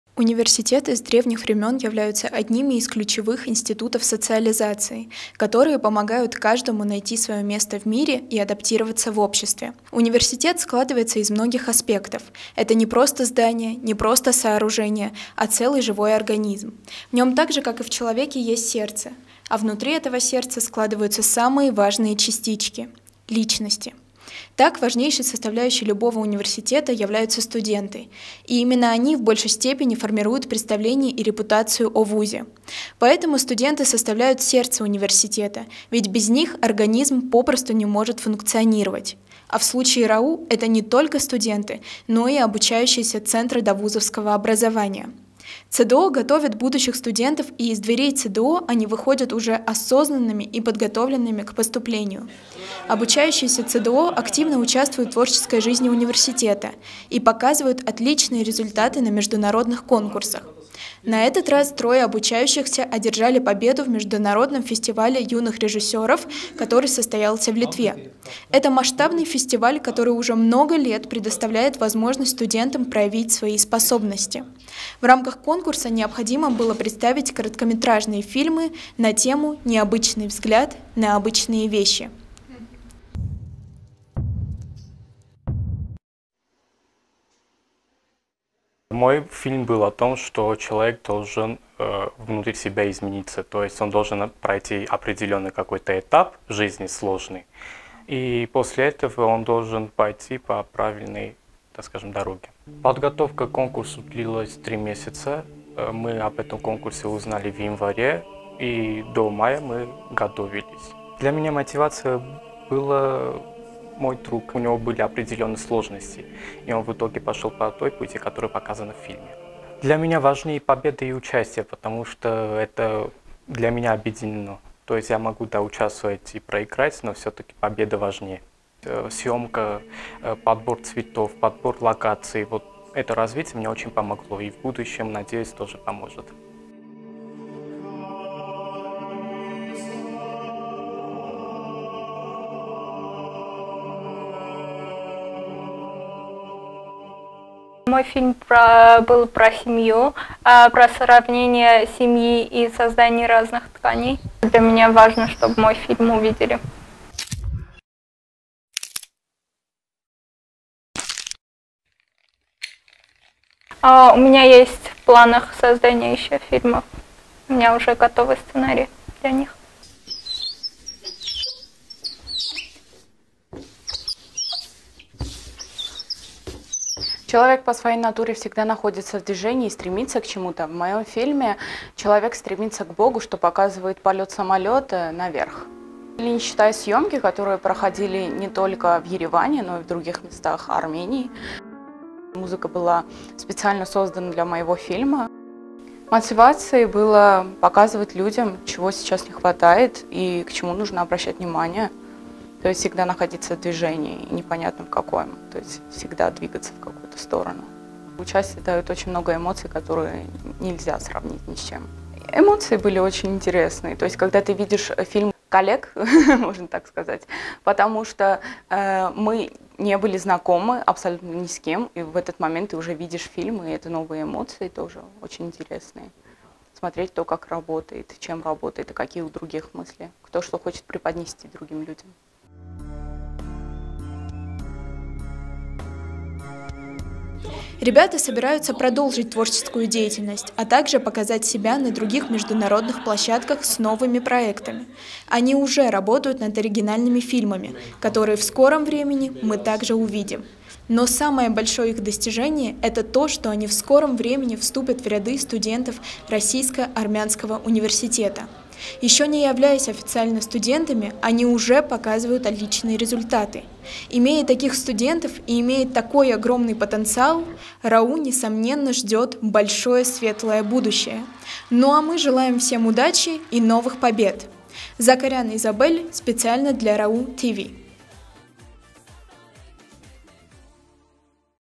Студенты ЦДО-победители Международного кинофестиваля
- Победитель всегда один?
- А вот и нет! Это утверждение опровергли наши яркие, амбициозные студенты ЦДО Российско-Армянского университета, которые уже являются победителями для самих себя. Трое обучающихся одержали победу в Международном кинофестивале, проходившем в Литве. Они смогли занять лидирующие позиции благодаря упорству, стремлению , а главное, благодаря вере в себя.
Верьте в себя и всё обязательно получится!